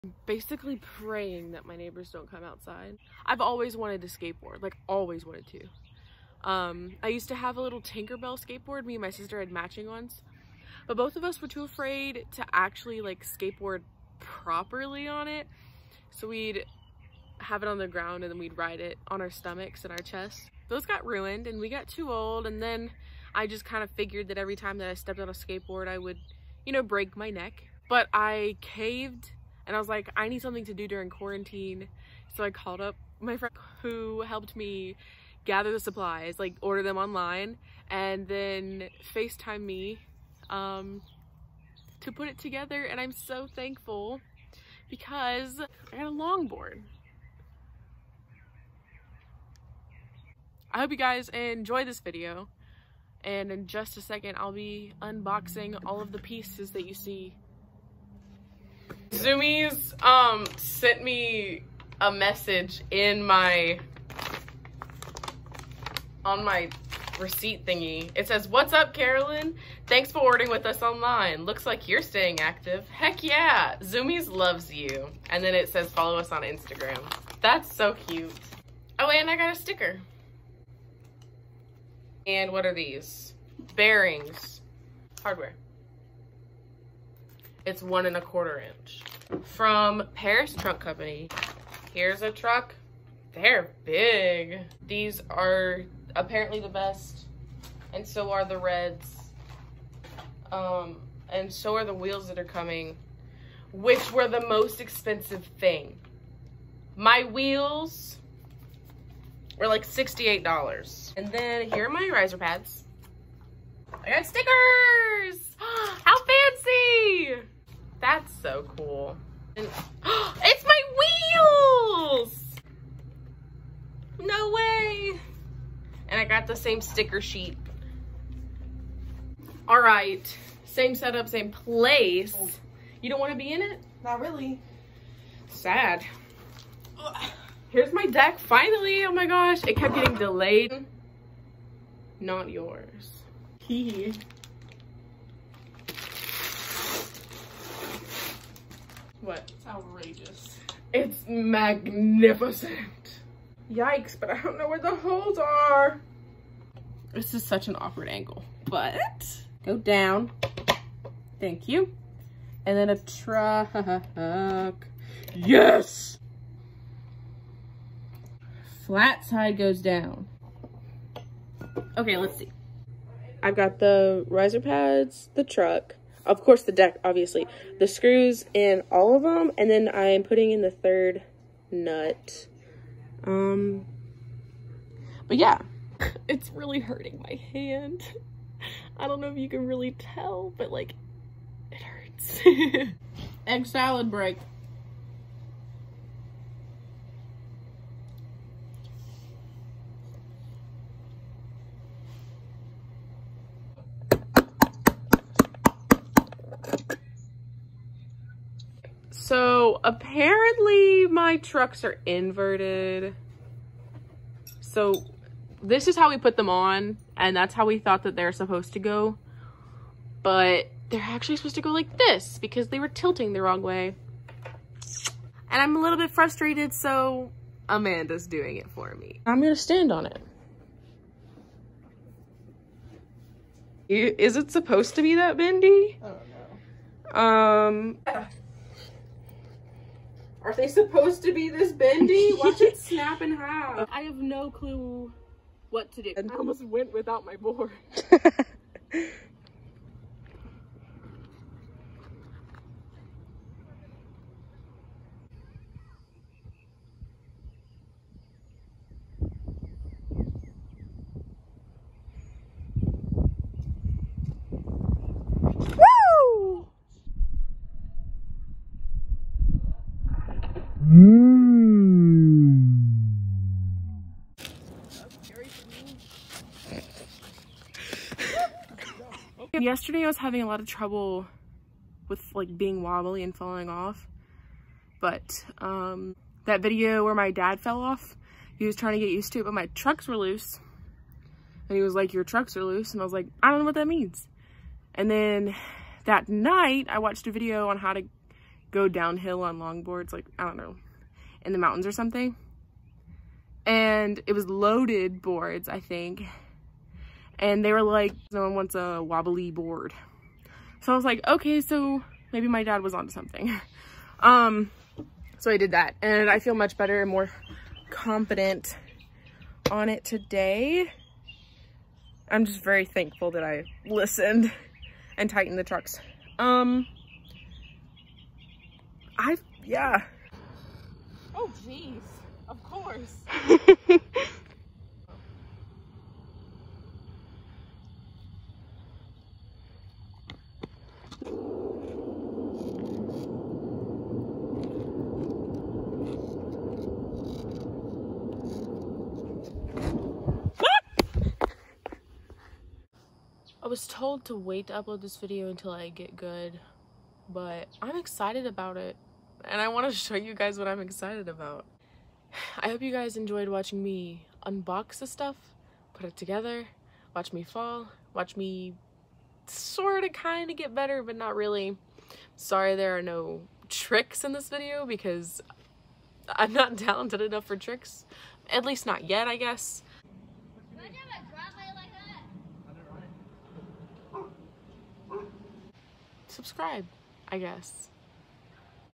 I Got a Longboard
I’ve been wanting to learn how to skate since I can remember, but I’ve always been too scared. I figured now was the perfect opportunity to learn and I can’t wait to get more comfortable on the board. A HUGE thank  you to my friend for helping me find the right parts and helping put them together. Also to help with tips I’ve been watching Hans Wouters, who is a very talented skater that y’all should check out

Wheels: Orangatang Caguama 85mm
Trucks: Paris V3 180mm 50° Polished
Riser Pads: Paris 1/4” Riser Pads
Hardware: Independent 1 1/4” black
Tool: Spitfire T3 Skate Tool
Bearings: Bones Reds Skateboard Bearings
Deck: FunBox 46” OG Dancer Longboard Skateboard Deck